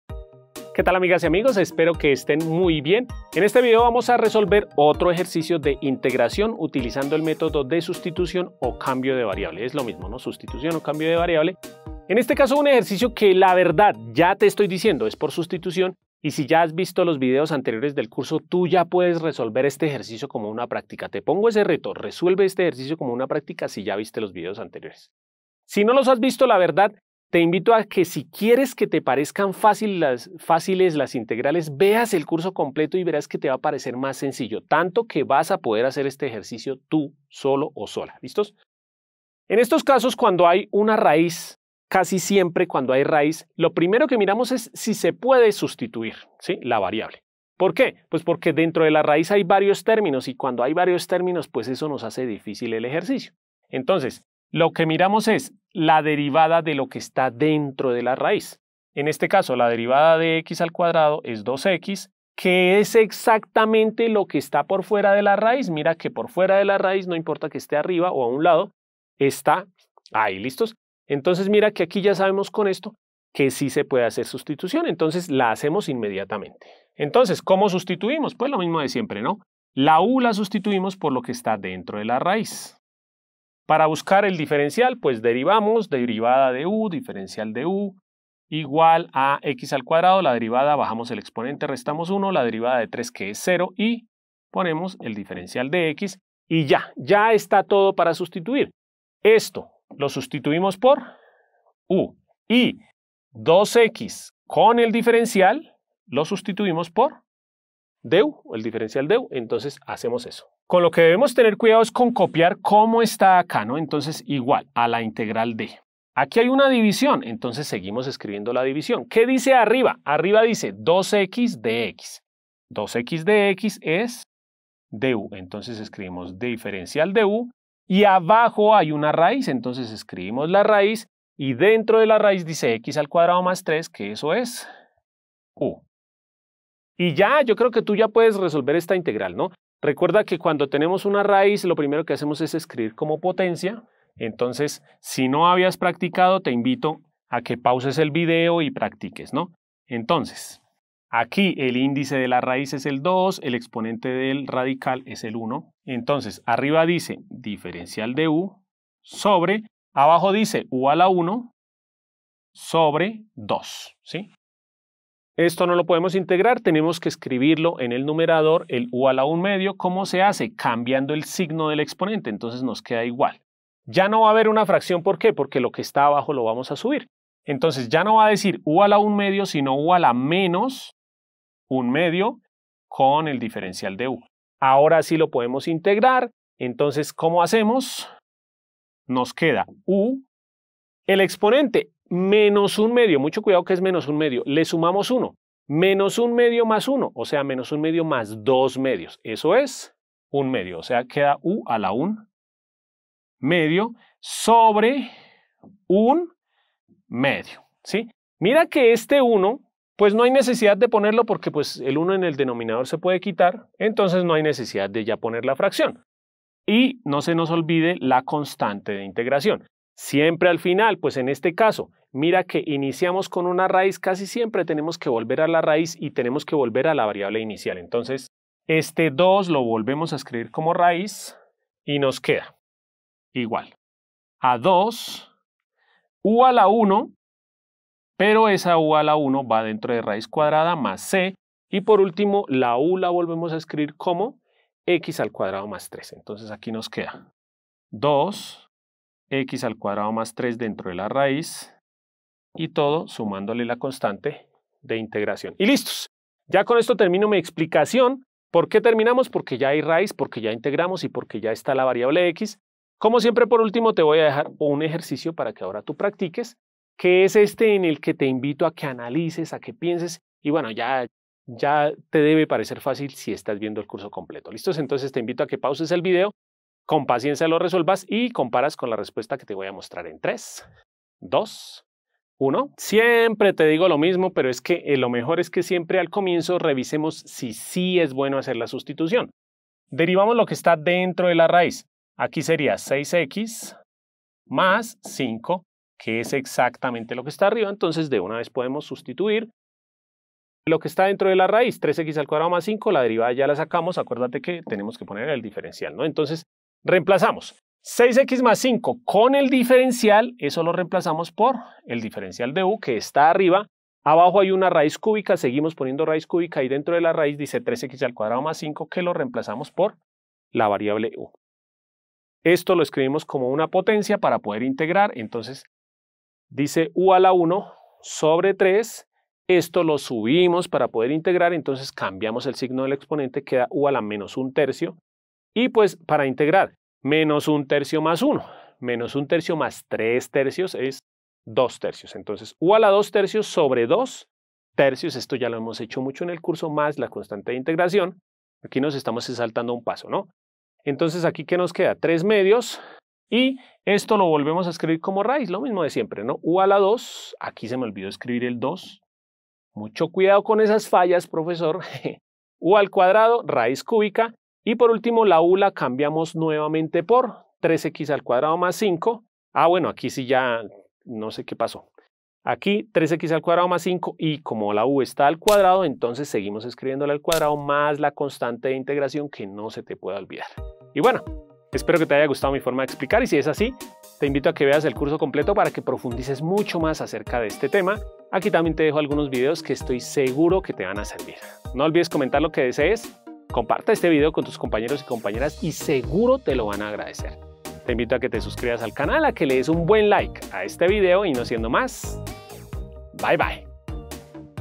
Integral de una raíz | Integrales por Sustitución - Cambio de variable | Ejemplo 11
Solución de integrales por sustitución o cambio de variable, en este caso cuando encontramos una raíz, que puede ser cuadrada, cúbica o con cualquier índice y que esa raíz está en el denominador de la fracción.

0:00 Saludo
0:10 Conceptos que debes saber
0:39 Solución del ejemplo
4:15 Ejercicio de práctica
6:20 Despedida y videos recomendados